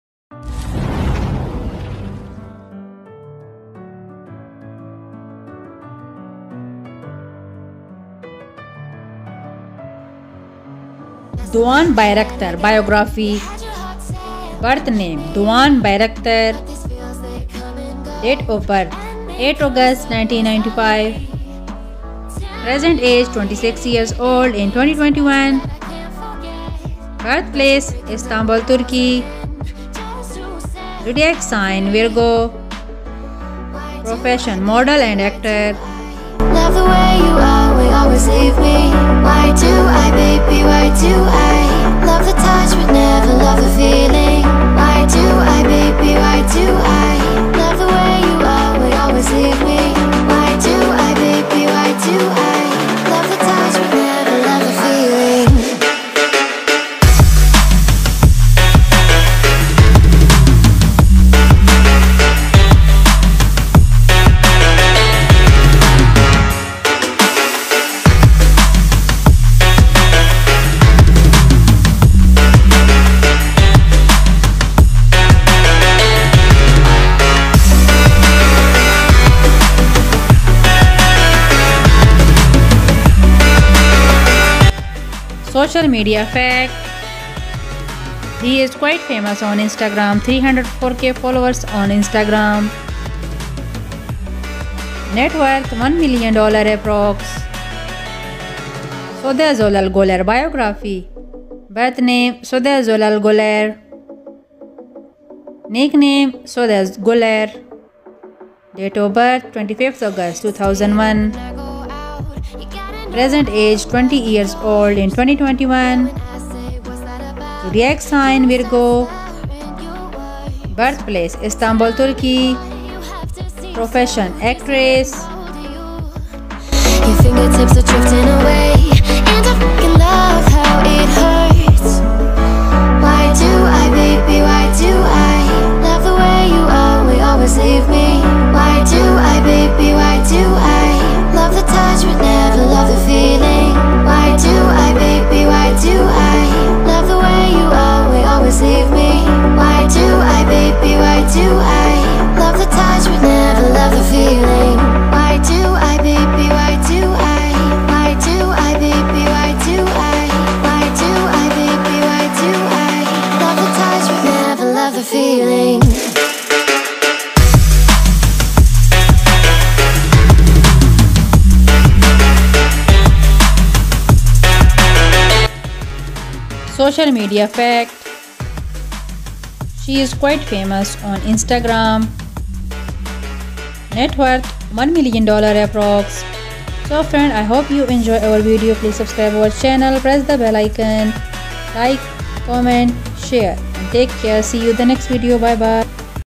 Doğan Bayraktar And Sude Zülal Güler (Masum Ve Güzel 2022) Real Life Partner 2022 & Age BY ShowTime
Dogan Bayraktar is an actor, known for Savasci (2017), Wolf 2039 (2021) and Hercai (2019).

Sude Zulal Güler is an actress, known for Kesif (2018), Ögrenci Evi (2021) and Seref Sözü (2020).